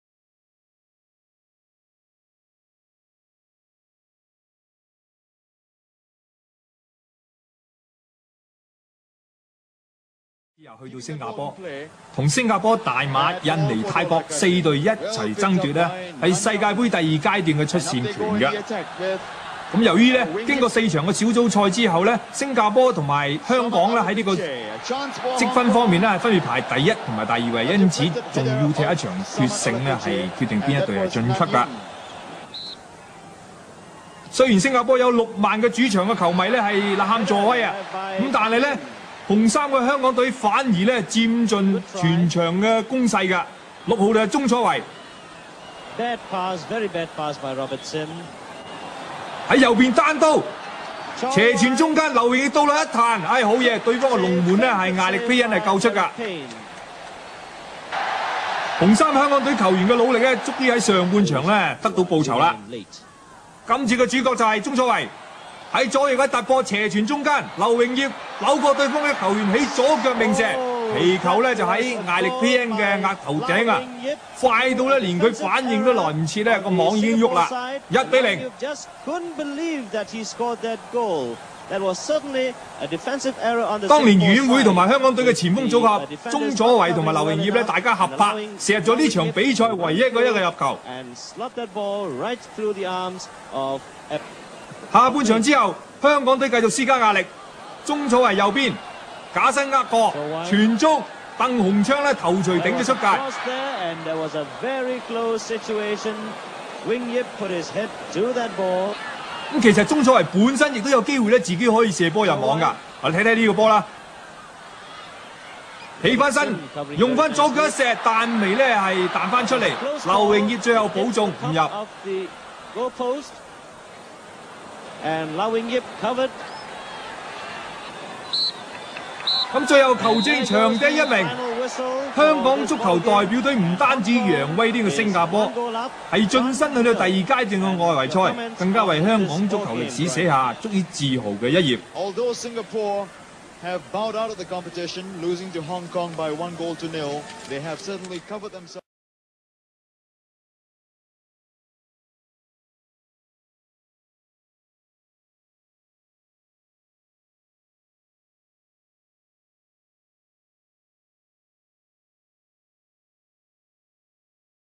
The Day A Girl Sobbed For The Lions in March 1977
Included a rare footage of the already demolished National Stadium of Singapore which was the setting of an epic match between Singapore and Hong Kong